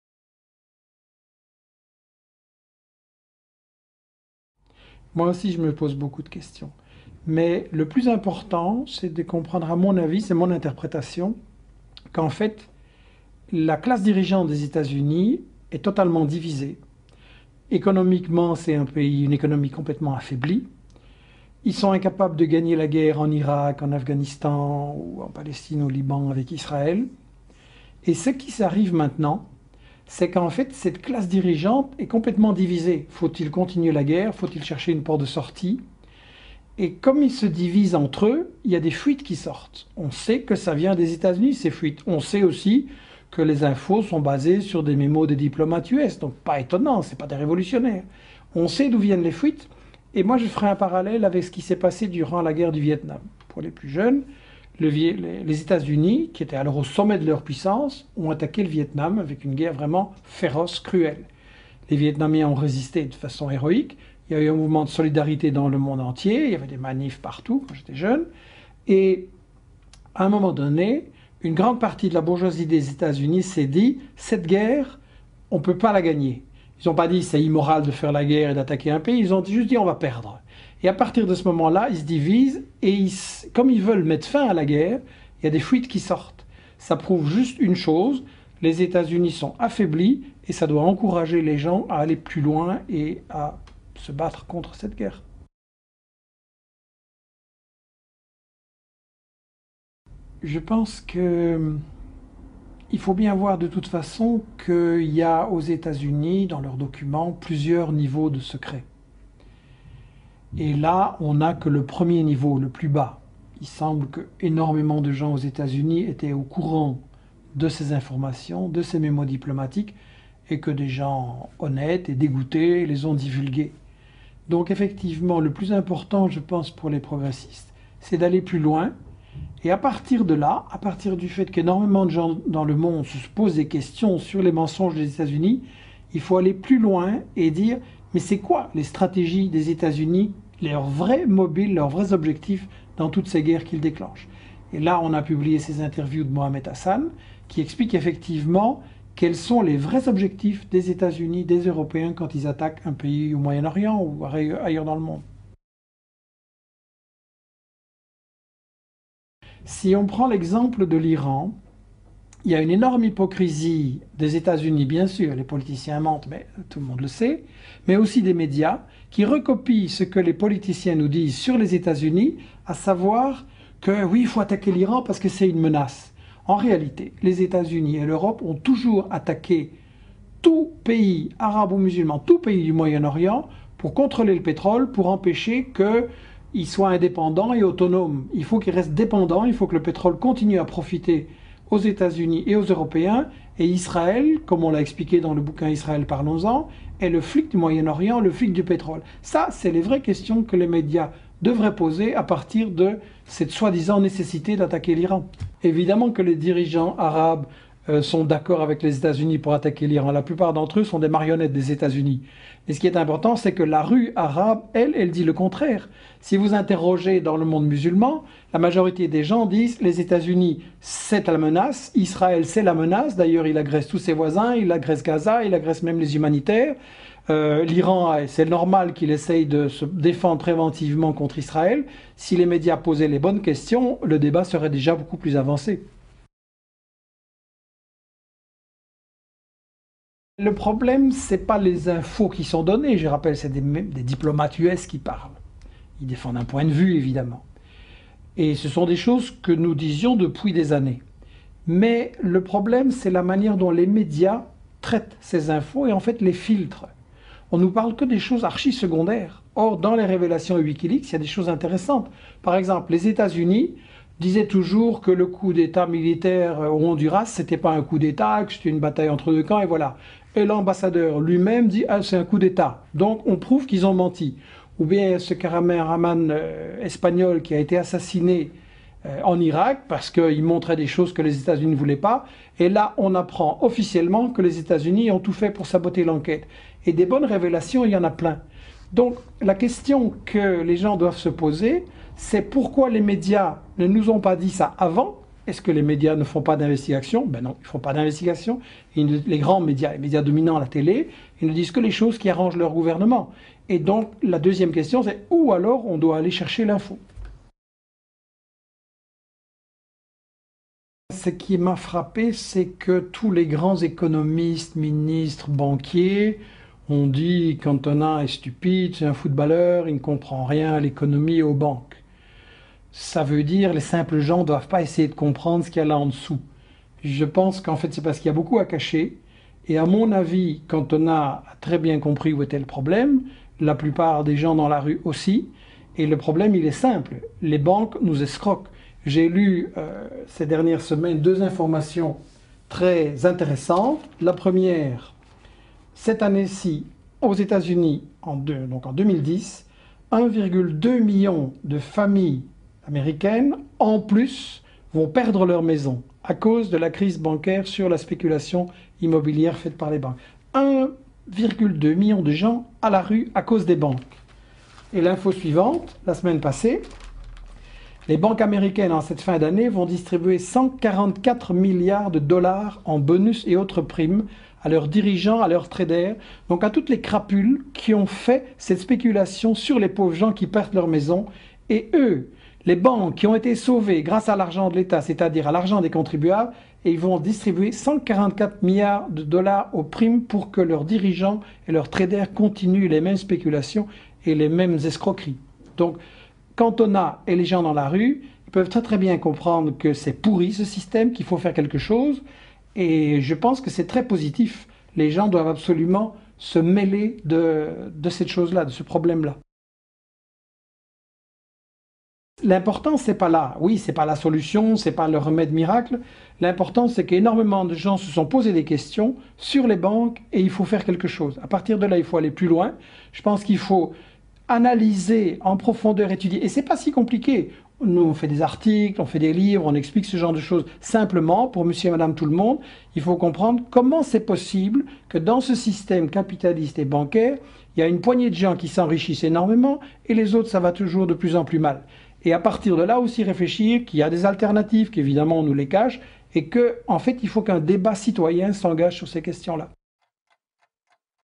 ウィキリークス情報漏洩 帝国の戦略を読め
ベルギ－人ジャ－ナリスト、ミシェル・コロン。２０１０年１２月。